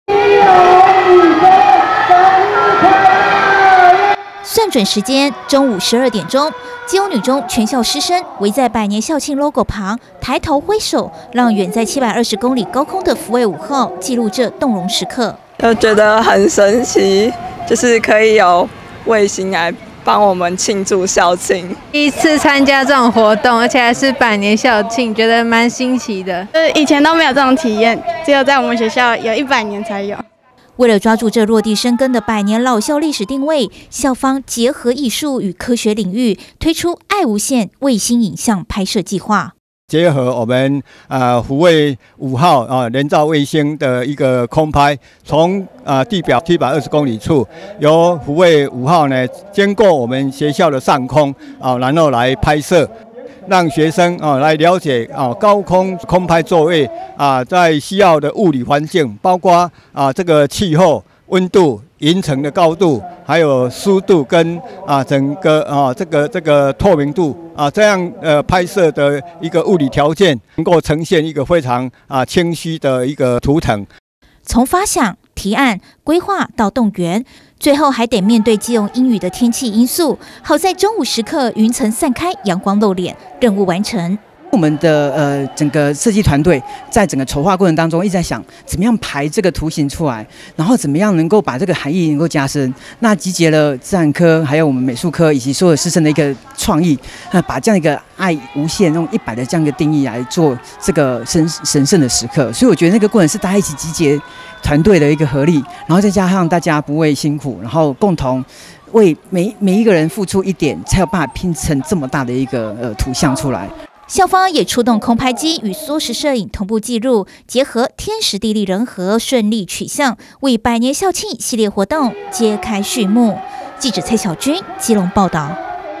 基女 百年|福衛五號拍攝基女百年校慶~無限i∞
(中嘉吉隆新聞 記者蔡曉君/基隆報導) 國立基隆女中為迎接明年的百年校慶，今天特舉辦一場師生史無前例的「愛無限衛星影像拍攝」計畫。與國家太空中心合作，以衛星福衛五號拍攝校地排字影像，記錄學校百歲生日的歷史軌跡，期許飛越 100，再造風華！

算準時間，中午12點整，基隆女中全校師生，圍在百年校慶logo旁，抬頭揮手，讓遠在720公里高空的福衛五號，記錄這動容時刻。

為了抓住這落地生根百年老校的歷史定位，校長陳明印指出結合藝術與科學領域，推出「愛無限衛星影像拍攝」計畫。

圖書館主任林宏維分享活動從發想、提案、規劃到動員，最後還得面對基隆陰雨的天氣因素，好在中午時刻雲層散開陽光露臉，任務完成！

校方也出動空拍機與縮時攝影同步紀錄，結合天時地利人合，順利取像，為百年校慶系列活動揭開序幕！

臉書追起來
Home 中嘉- 吉隆有線電視

趣聞不漏接
吉隆官網(中嘉新聞)

喜歡看電視?